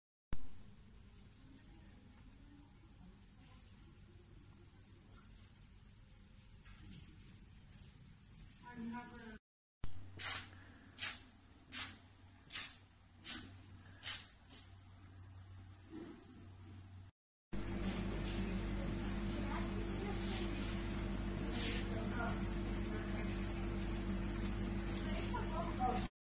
Κακοκαιρία - Αεροδρόμιο Σαντορίνης - GRTimes.gr
«Πνίγηκε» στο χαλάζι το αεροδρόμιο της Σαντορίνης